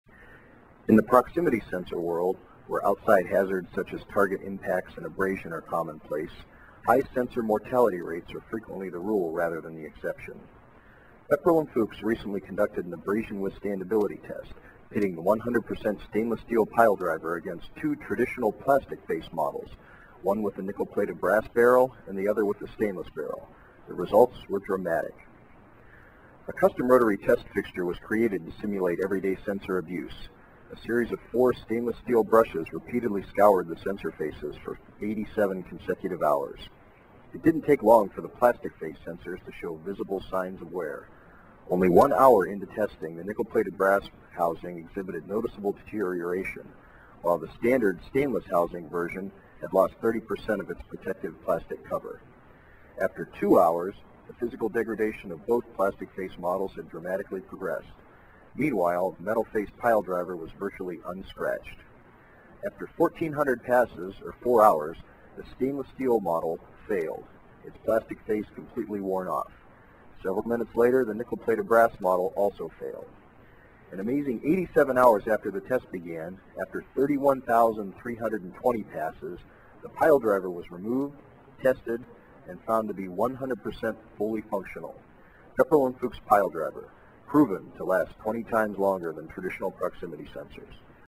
Pile Driver Inductive Proximity Sensor Abrasion Test
- Pepperl+Fuchs recently conducted an abrasion test, pitting our 100% stainless steel Pile Driver inductive proximity sensor against two traditional plastic face models. After four hours, the traditional inductive proximity sensors failed. Pile Driver withstood 87 hours of stainless steel brushes scouring its surface, and was still 100% functional. Pile Driver lasted 20 times longer than traditional proximity sensors.